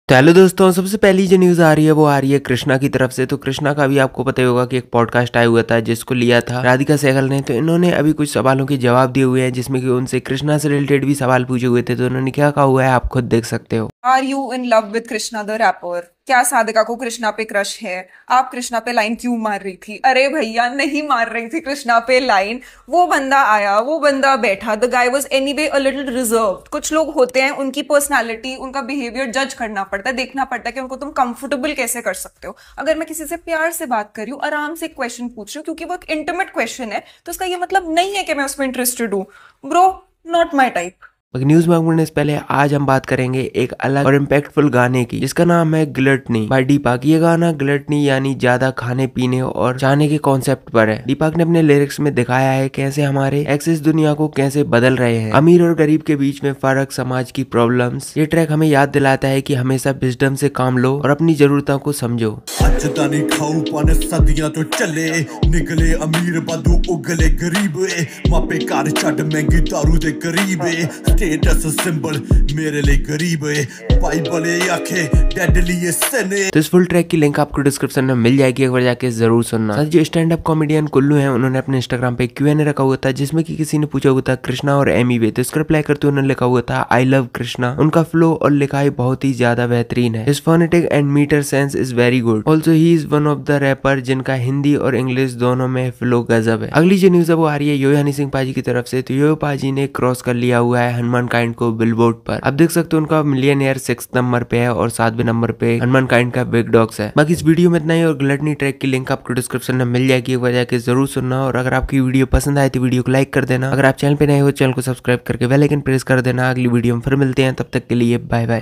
KULLU REPLY ON EMIWAY OR KR$NA | SADHIKA SEHGAL LOVE WITH KR$NA? | HONEY SINGH SURPASSES HANUMANKIND
News Credit :-